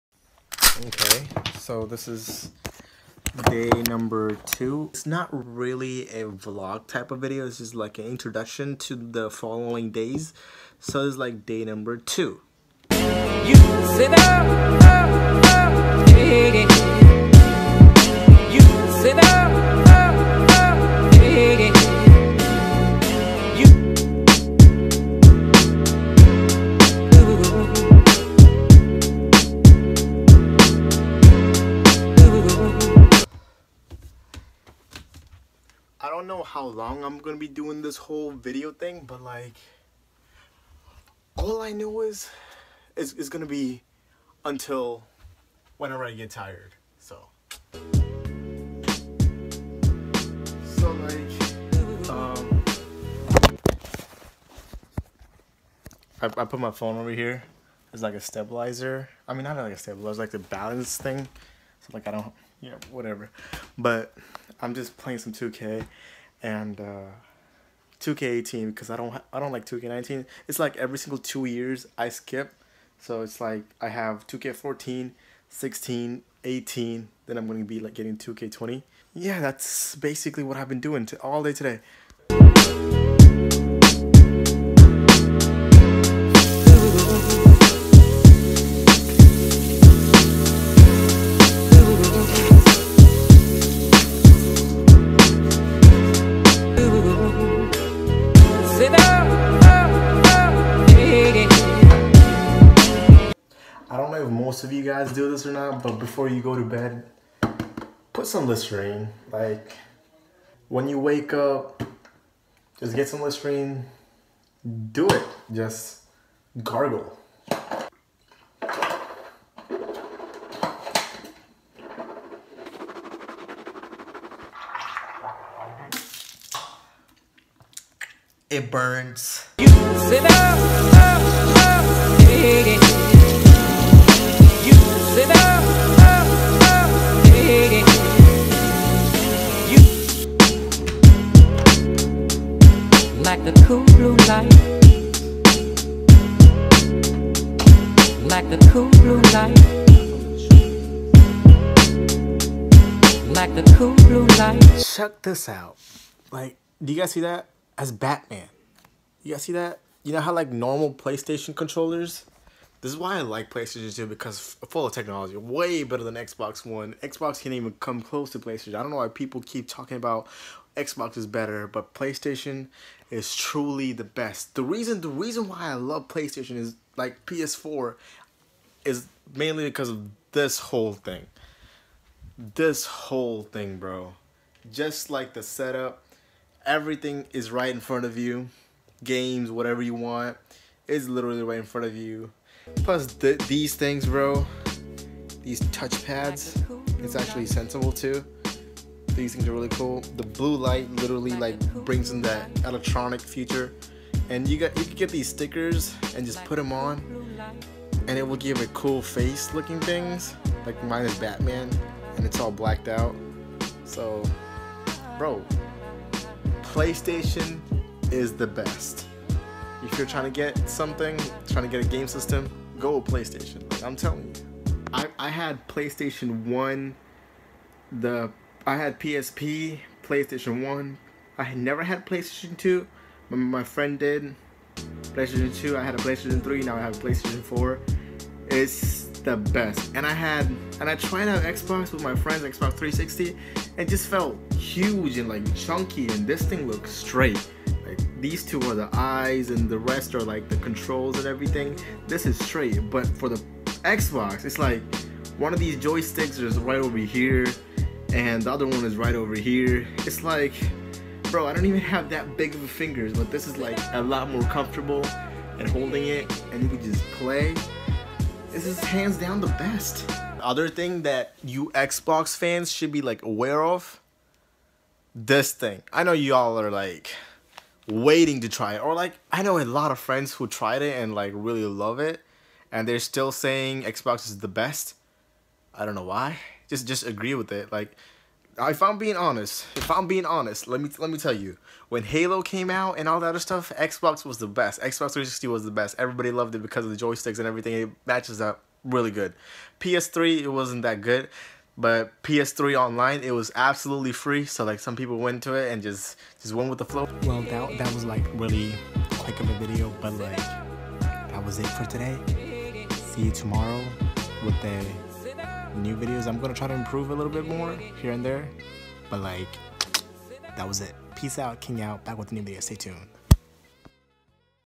This is why PS4 is the best gaming system!!!
I personally think PS4 is way better than Xbox one.
This is the reason why I think PlayStation is the best system out there...

Rise like a king
Dream chasers